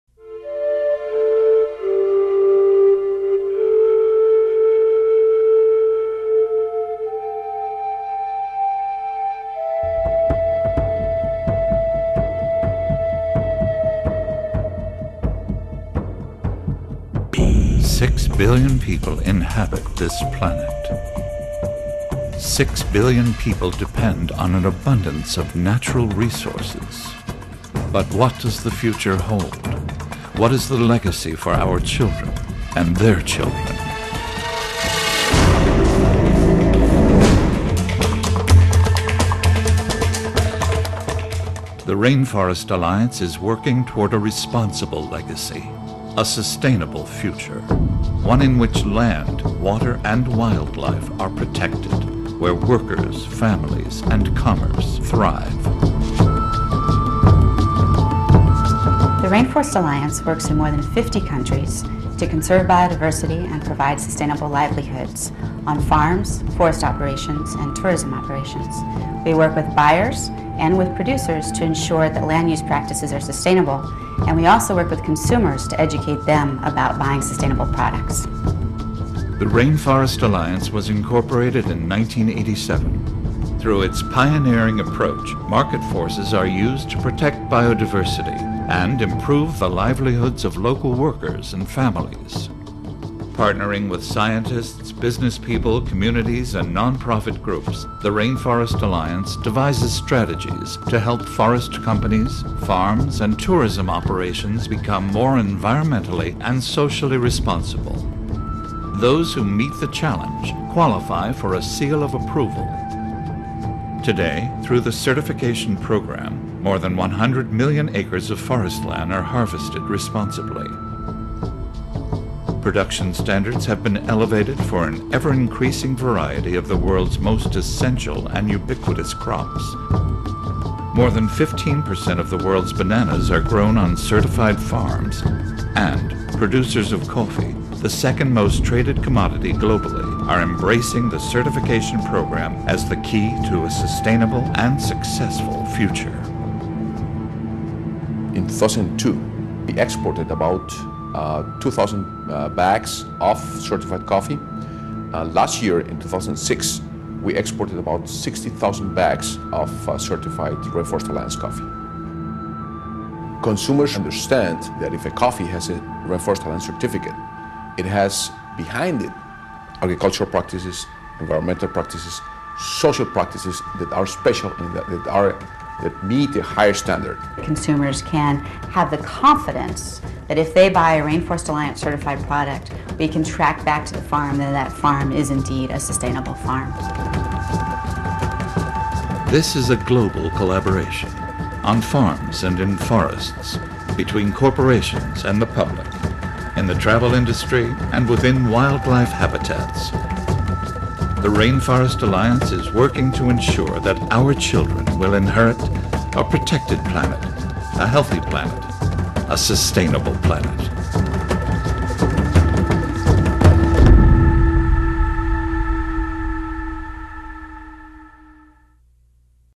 Rainforest Alliance
As a growing human population impacts ecosystems around the world, we see certification as an increasingly important tool for driving continual positive change. It provides an effective method for ensuring that natural resources are managed for the long term.